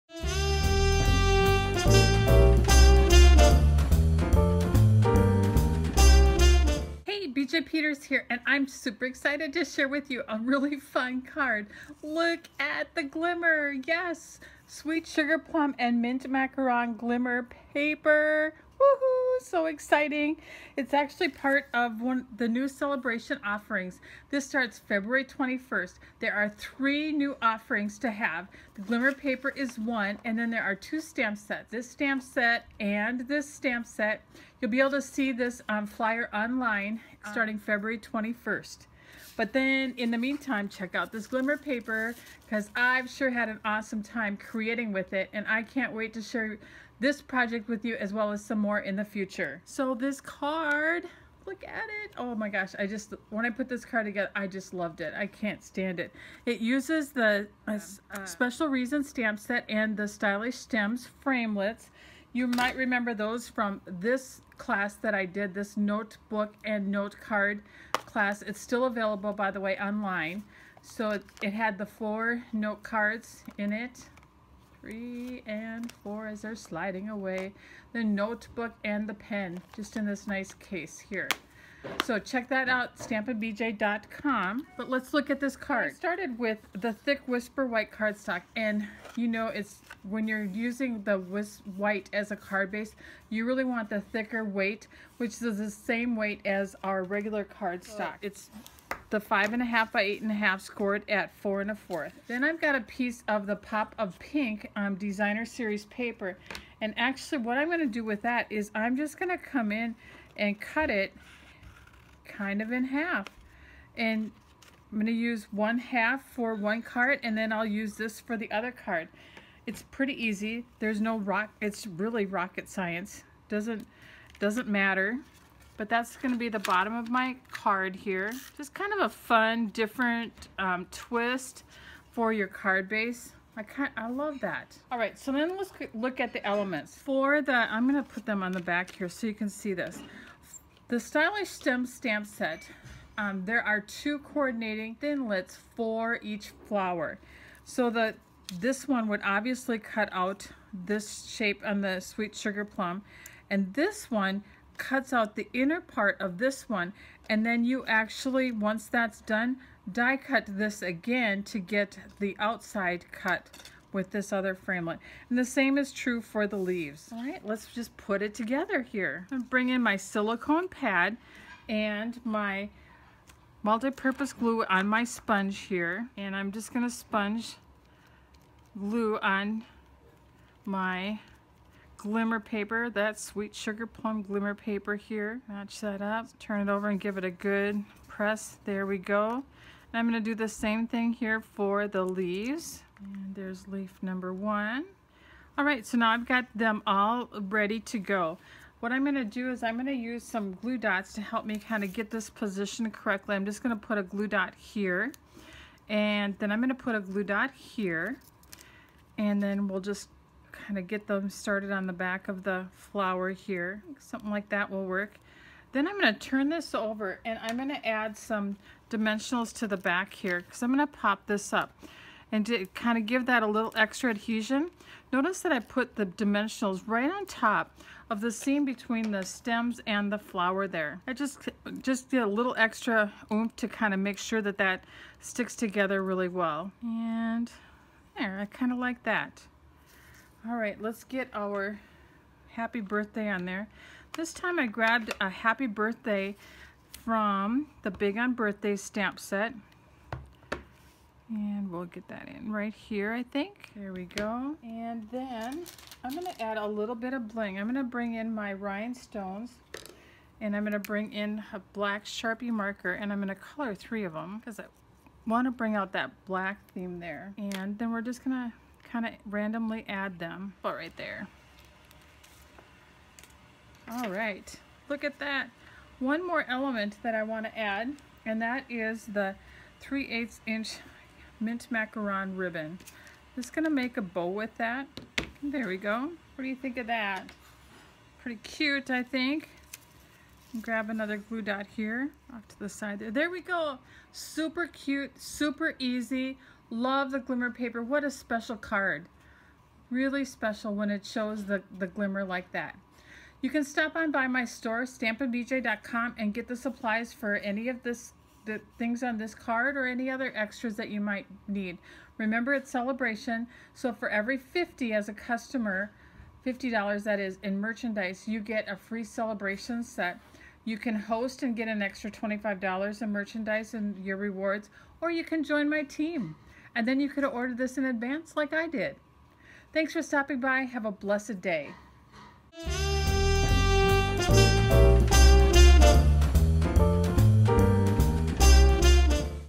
Glimmerific Special Reason
You will LOVE how the new Glimmer Papers available during Sale-a-Bration from Stampin' Up!  The Stylish Stems thinlits truly are perfect for this glimmer paper!